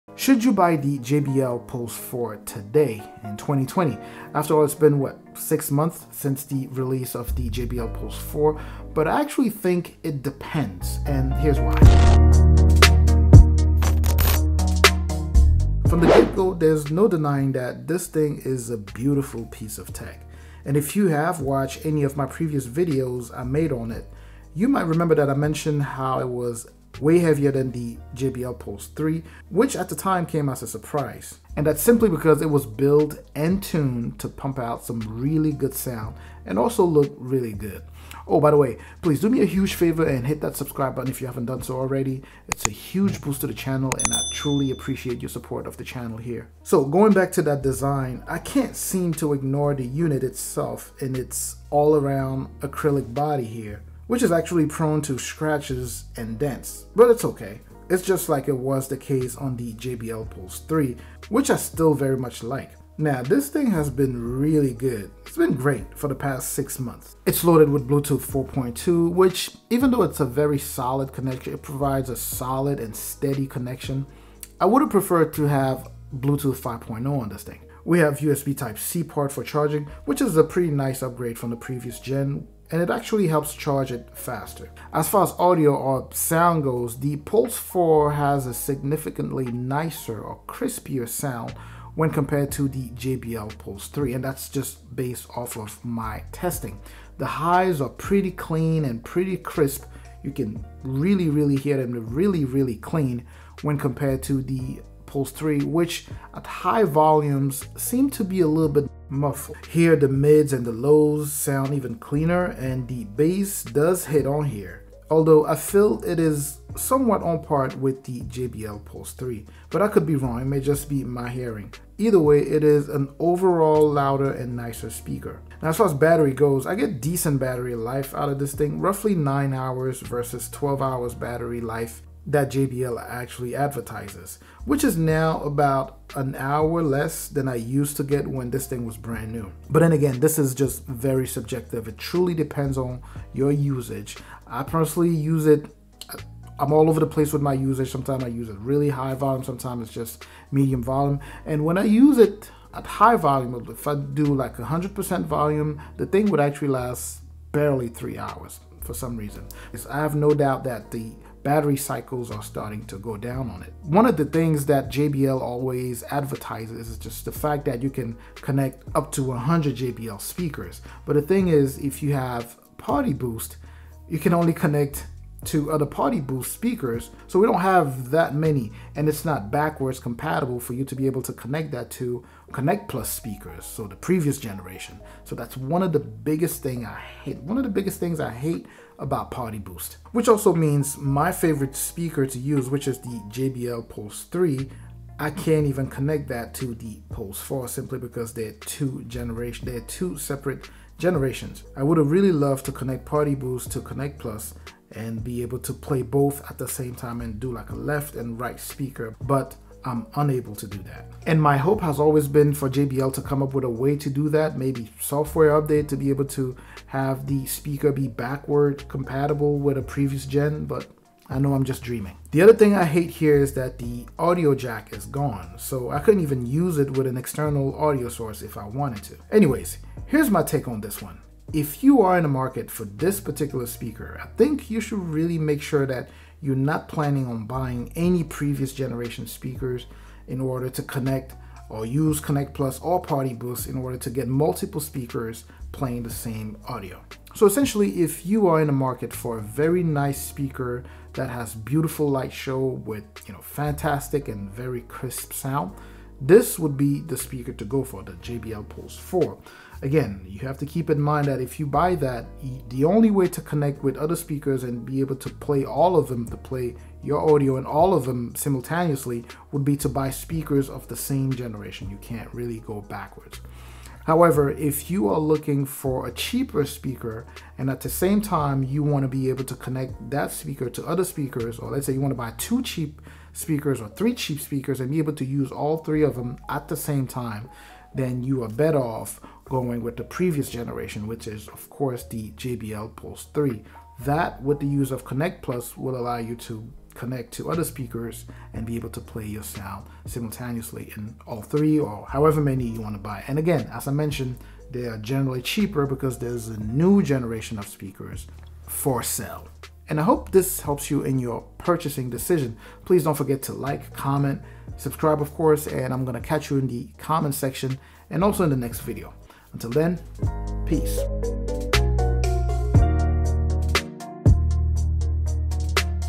JBL Pulse 4 vs Pulse 3 in 2020- Should You Upgrade?
In this video I give my take on whether you should upgrade to the JBL Pulse 4 or buy a Pulse 4, as opposed to the JBL Pulse 3 bluetooth speaker.
The Pulse 4 would be a good idea if you are not planning on using it as a Connect+ speaker and if you are just looking for the latest and the coolest of JBL.
The Pulse 3 on the other hand provides more value and you can connect to a variety of JBL speakers, using Connect+.
JBL does not seem to provide a way to make the Pulse 4 backwards compatible from what I can tell. Please share your findings if you have heard anything about this in the Comment section.

PULSE 4:

PULSE 3:

  *** HERE IS A LIST OF THE GEAR I USED IN THIS VIDEO ***
CAMERA:

MIC BOOMPOLE: